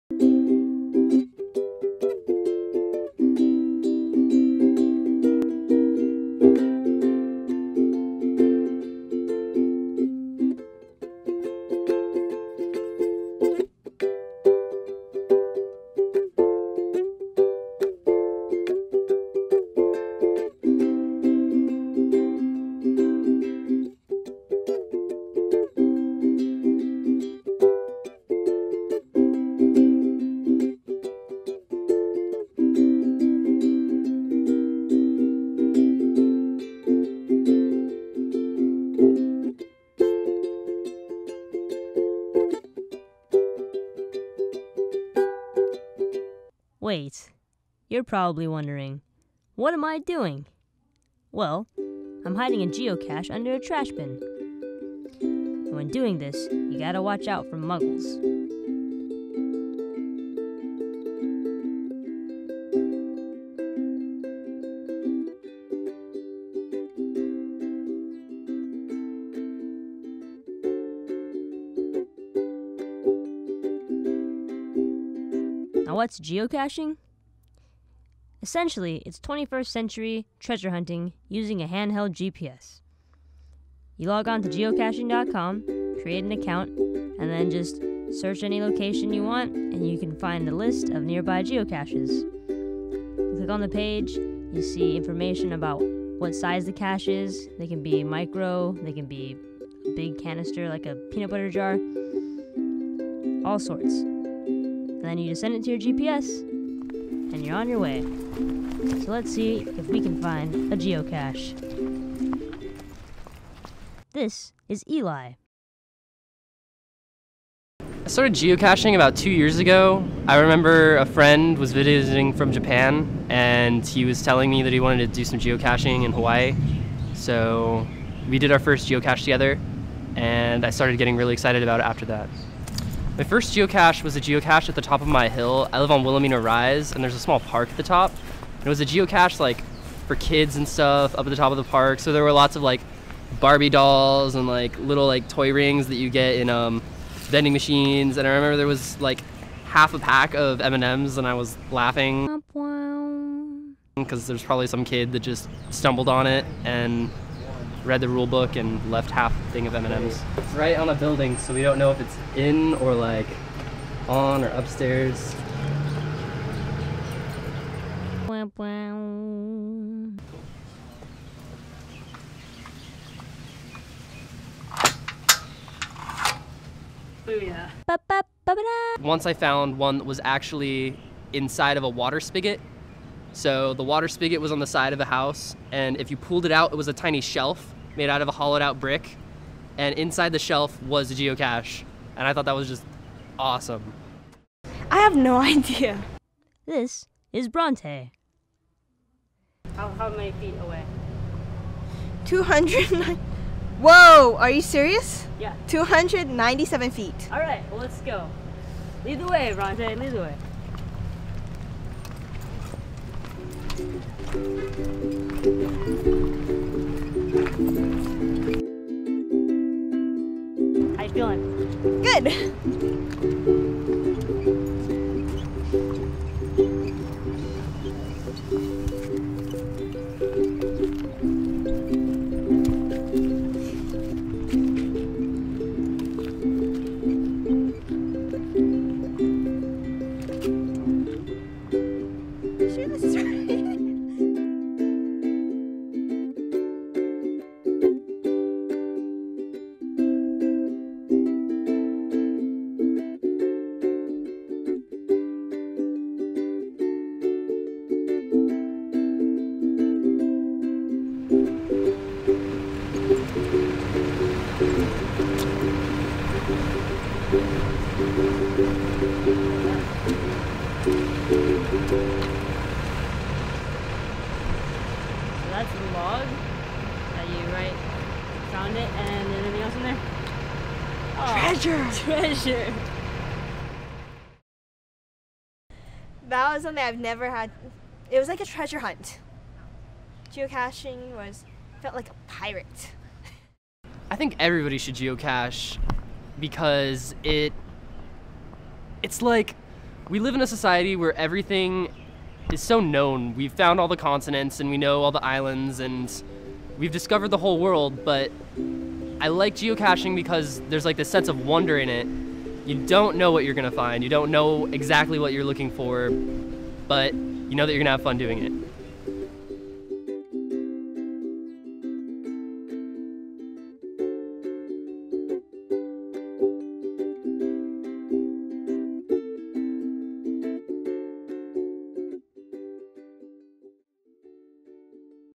Caching In
A short film about geocaching: modern-day treasure hunting.